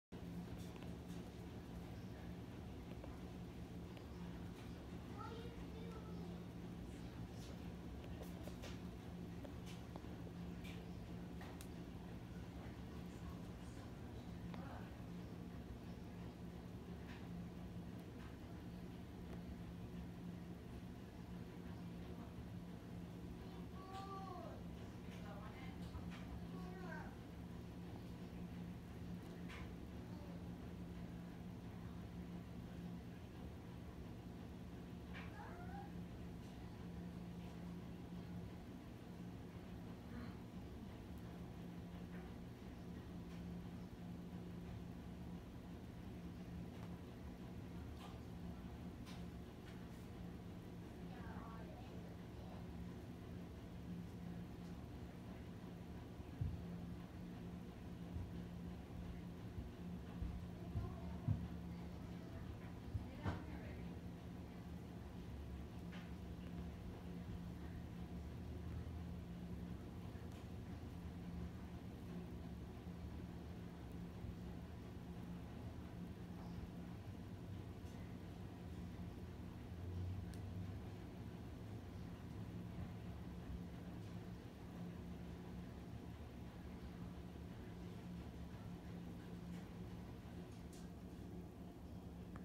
Kitty battle
Two kittens playing and attacking each other